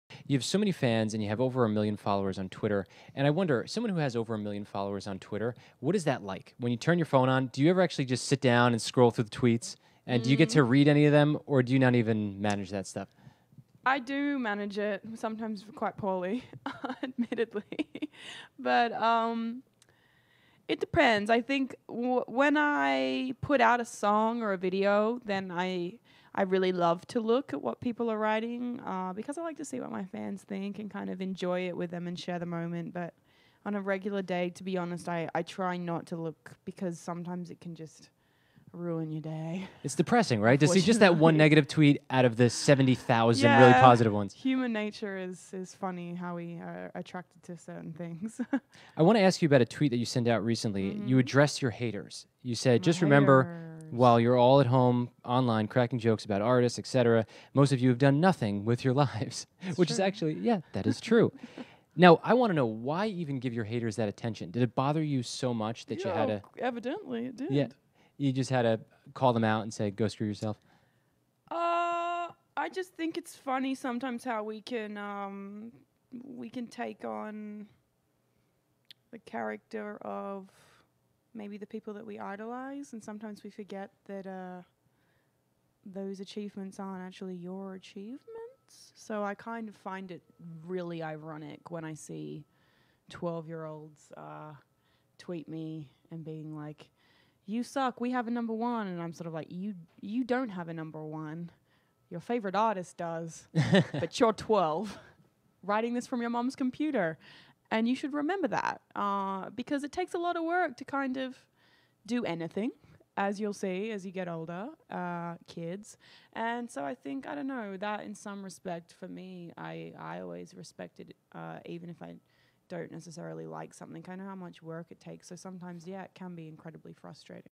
Iggy Azalea Talks Social Media Haters in 103.3 AMP Radio Interview
Australian rapper Iggy Azalea visited Boston's 103.3 AMP Radio (a Radio.com station) and discussed why she avoids using Twitter and other social networks on a regular basis.

Iggy Azalea Talks Origin Of Her Stage Name & Rise To Fame